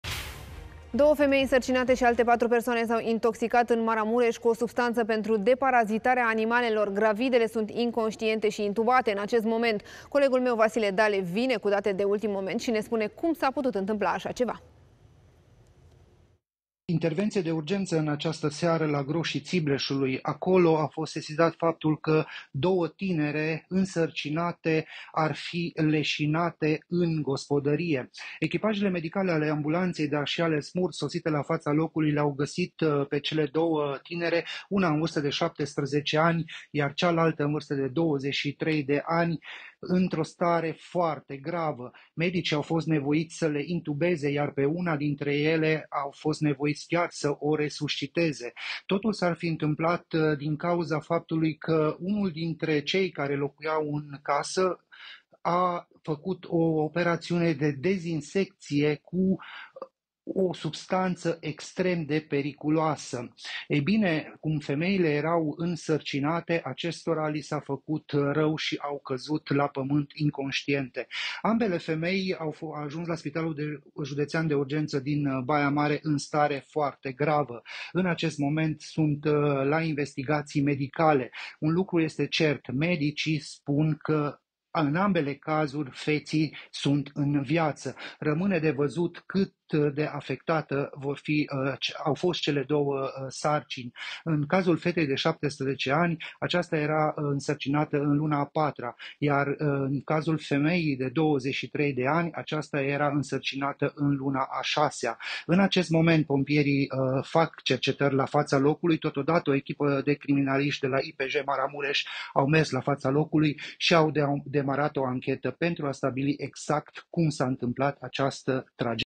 Două gravide au ajuns în stare gravă la spital, după ce s-au intoxicat cu o substanţă periculoasă, î
Intervenție de urgență în această seară la Groși Țibleșului. Acolo a fost sesizat faptul că două tinere însărcinate ar fi leșinate în gospodărie.

Echipajele medicale ale ambulanței, dar și ale SMURD sosite la fața locului le-au găsit pe cele două tinere, una în vârstă de 17 ani, iar cealaltă în vârstă de 23 de ani, într-o stare foarte gravă.